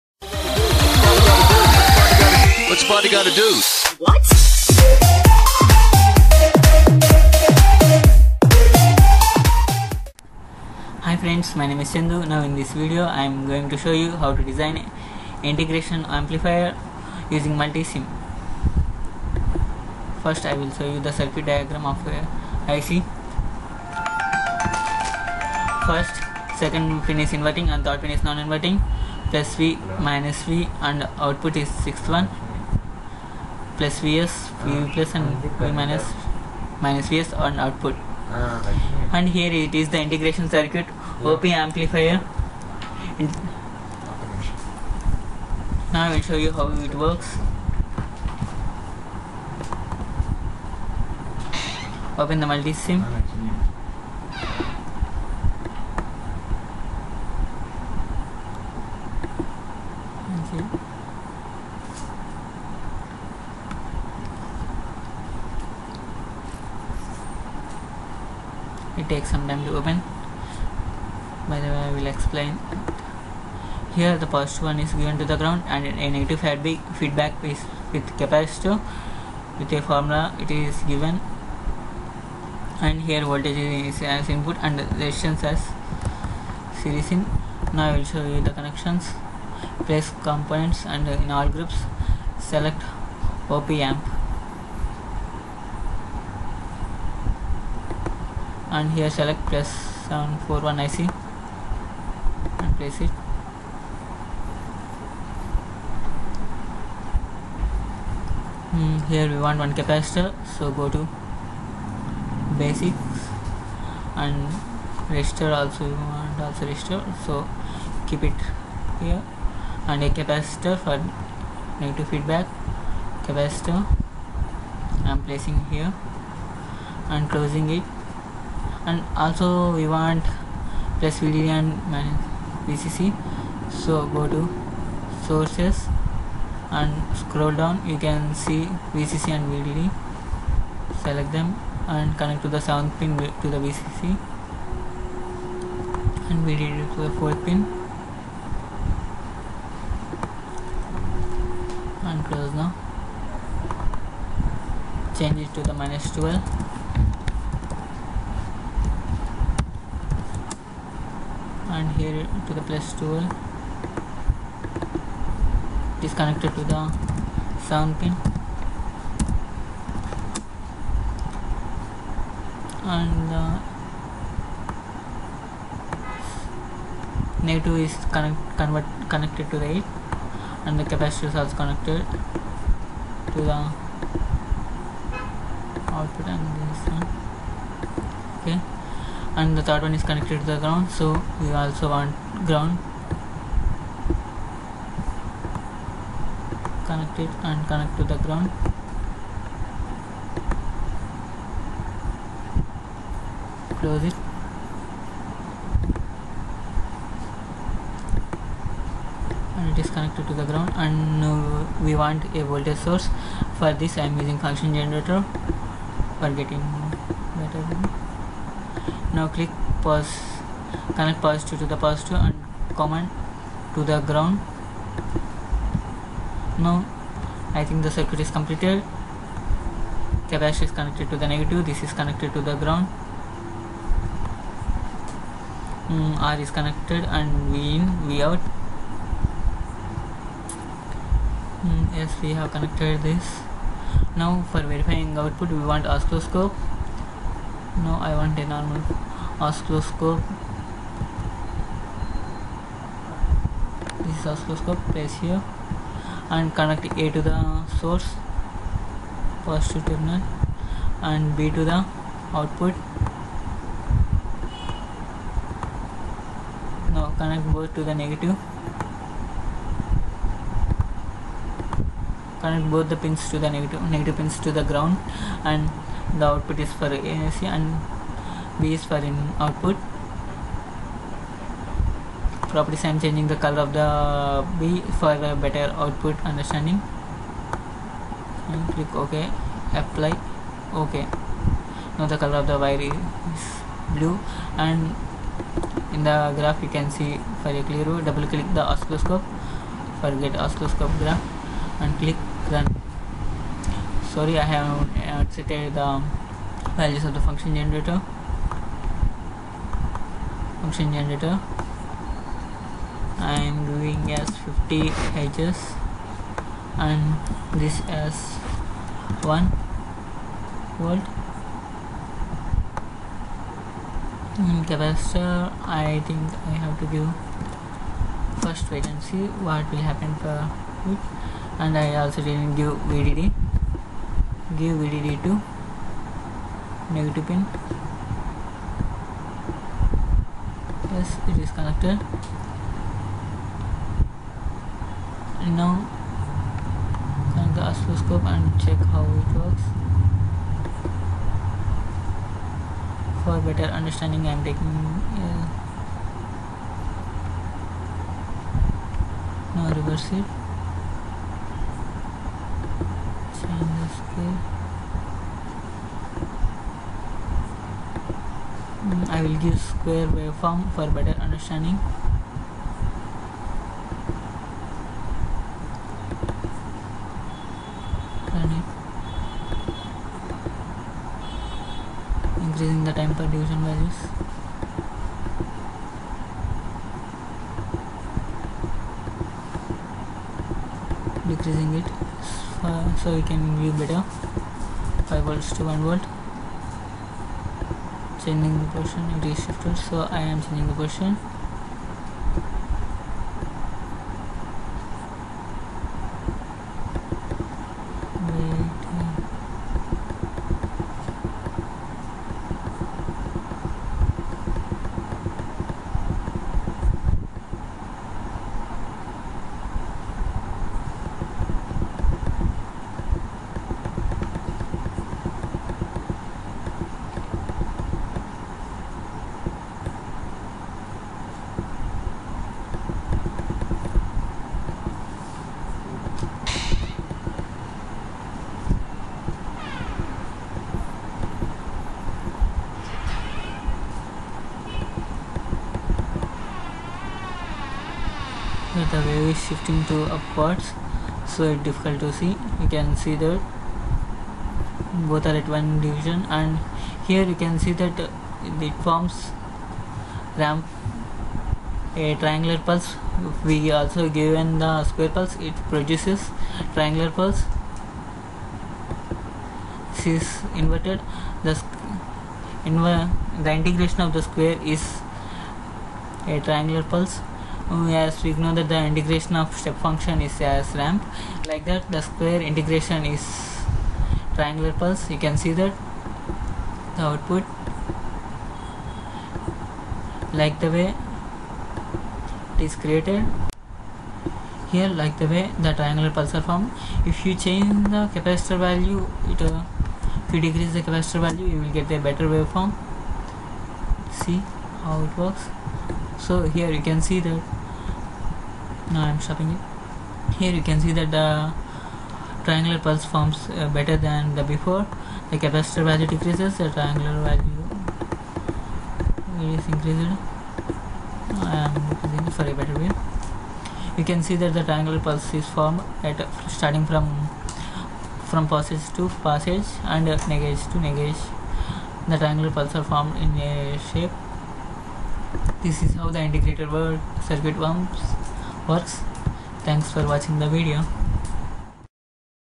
Integration op amplifier using multisim
The operational amplifier integrator is an electronic integration circuit. Based on the operational amplifier (op-amp), it performs the mathematical operation of integration with respect to time; that is, its output voltage is proportional to the input voltage integrated over time.